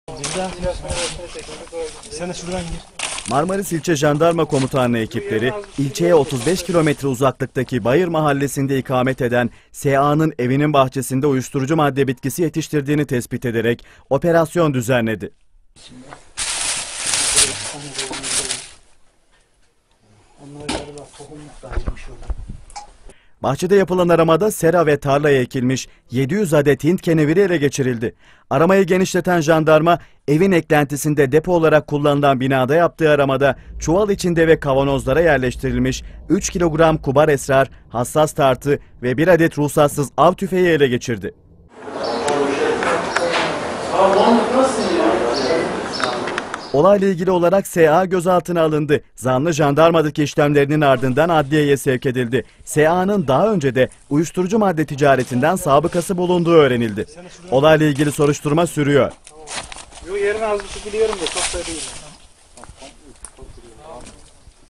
UYUŞTURUCU TARLASINA OPERASYON
Marmaris’te bir evin bahçesine operasyon düzenleyen jandarma ekipleri, 700 kök Hint keneviri ile 3 kilogram kubar esrar maddesi ele geçirdi. Olayla ilgili bir kişi gözaltına alındı.